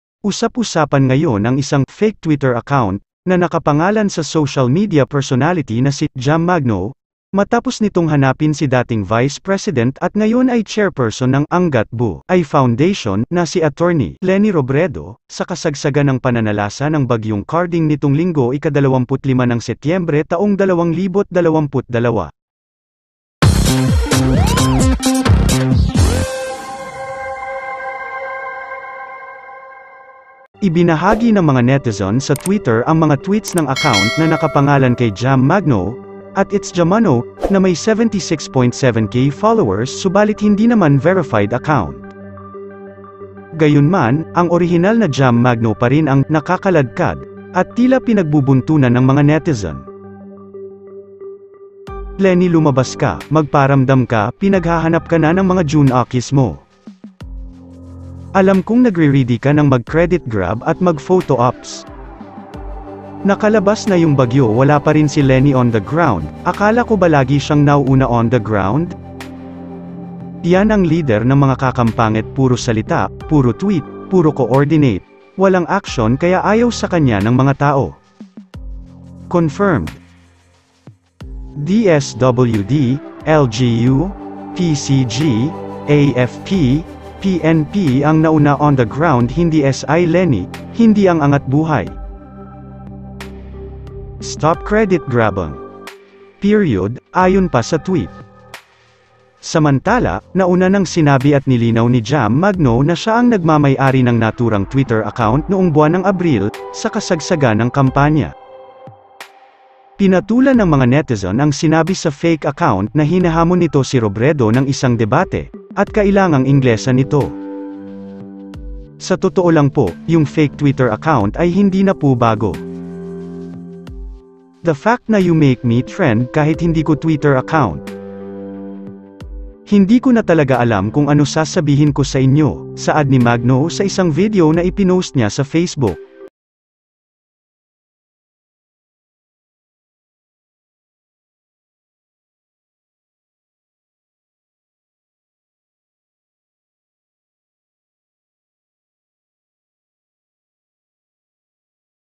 LENI LUMABAS KA! Saad ni Jam magno sa post neto sa twitter na hinahanap Ang dating vise Presidente
Usap-usapan ngayon ang isang “fake Twitter account” na nakapangalan sa social media personality na si “Jam Magno” matapos nitong hanapin si dating Vice President at ngayon ay chairperson ng “Angat Buhay Foundation” na si Atty. Leni Robredo, sa kasagsagan ng pananalasa ng bagyong Karding nitong Linggo, Setyembre 25, 2022.